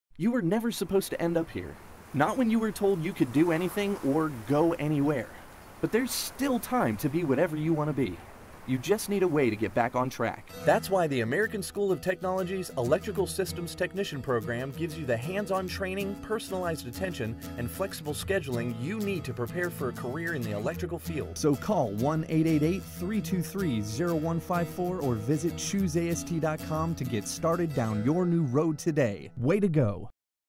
Electronic Systems Technician
AST utilizes a hands-on approach to prepare students for employment in various EST positions in the field. Start your career training TODAY!